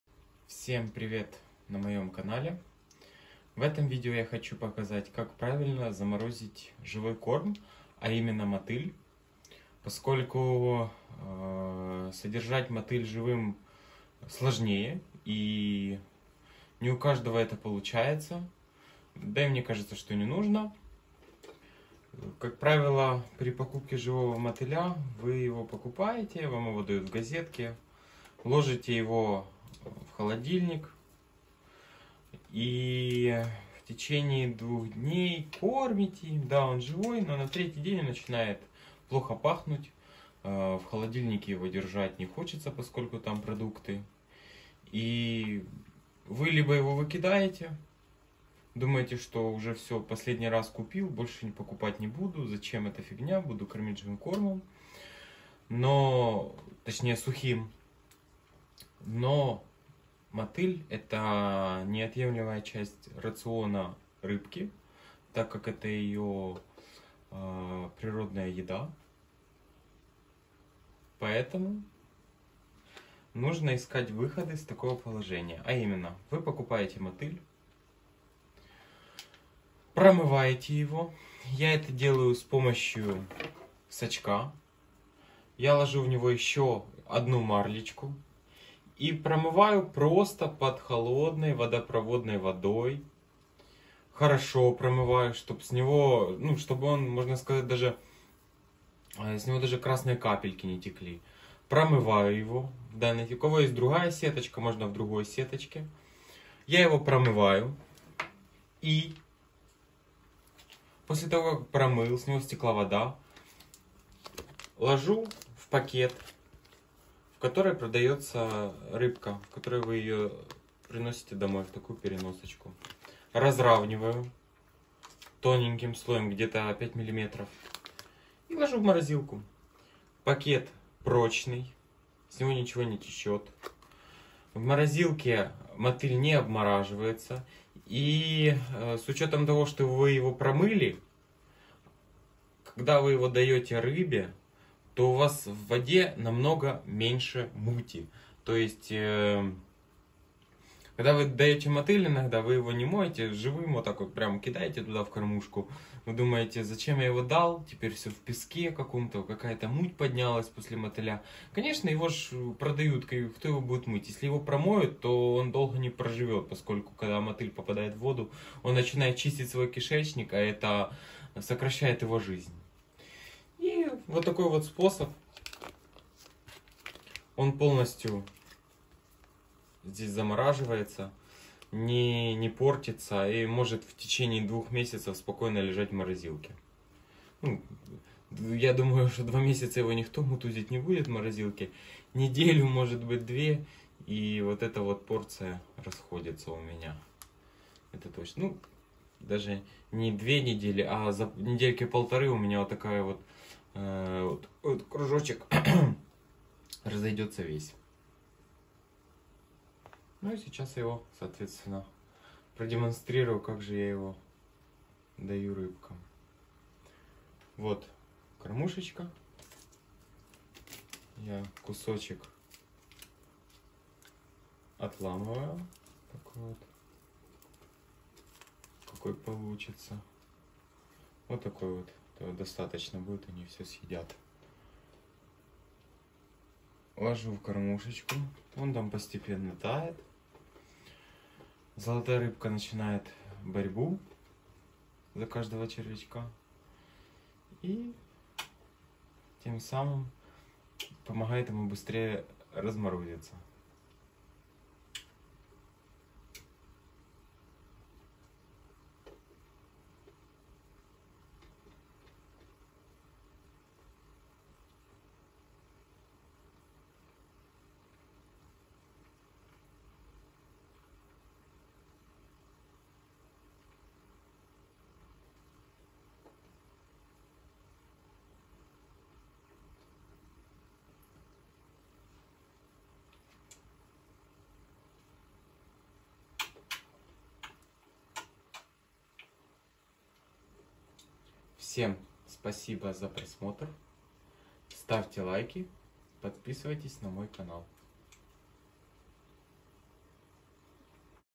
Как ПРАВИЛЬНО заморозить мотыль для аквариумных рыбок.
В этом видео я покажу надежный способ заморозки мотыля.